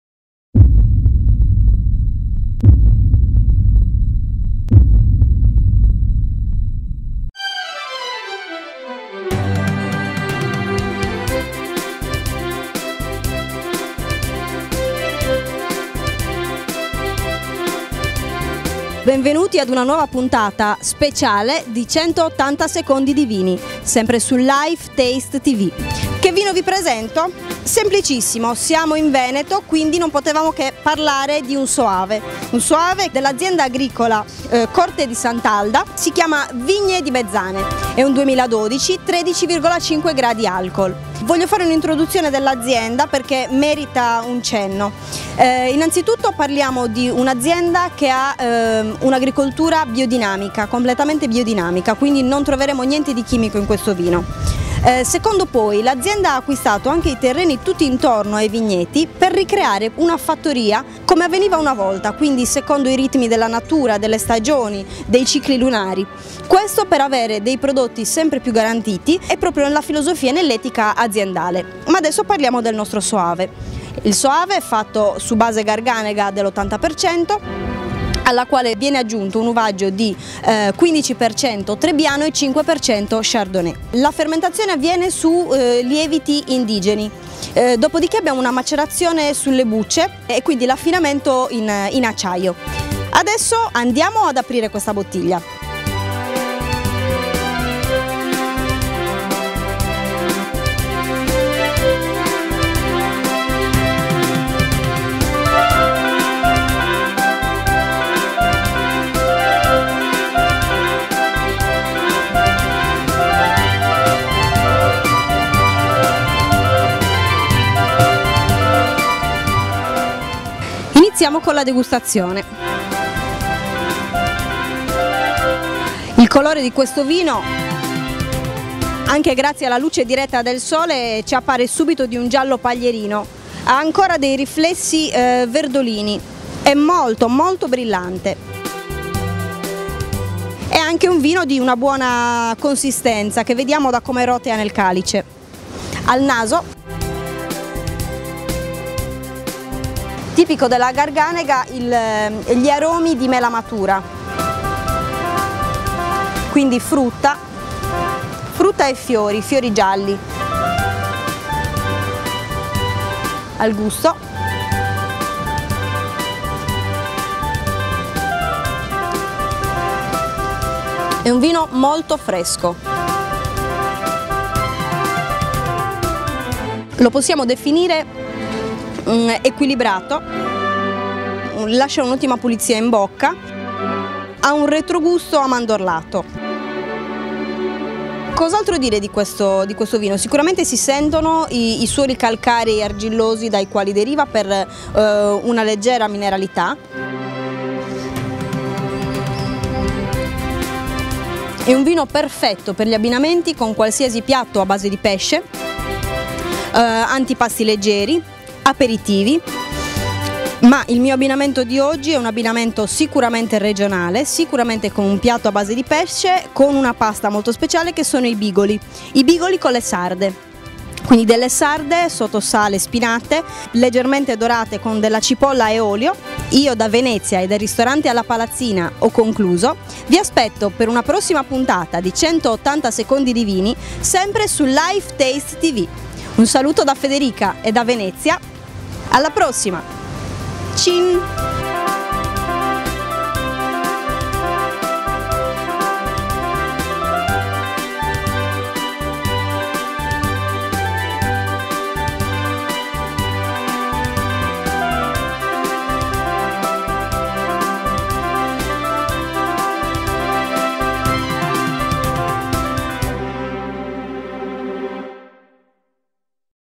180" DiVini | Azienda Agricola Corte Sant'Alda | Vigne di Mezzane 2012 | Soave d.o.c.
180" DiVini - Programma dedicato alla video-degustazione di vini nazionali e internazionali, condotto dalla Sommelier Federica Capobianco.

In questa puntata Federica vi presenta : Azienda Agricola Corte Sant'Alda | Vigne di Mezzane 2012 | Soave d.o.c.